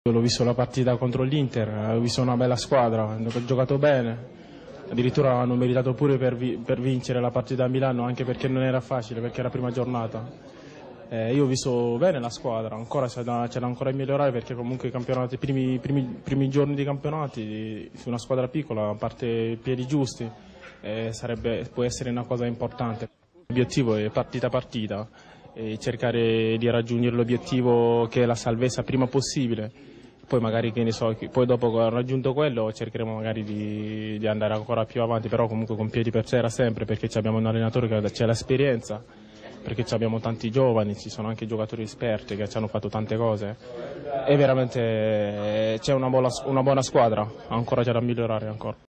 Intervista a Souleymane Diamoutene
Intervista a Souleymane Diamoutene (Difensore del Bari), presentato alla stampa, il difensore ha vestito anche la maglia del Onze Creatures(Mali),Lucchese, Perugia, Lecce, Roma, e conta anche delle presenze in nazionale del Mali.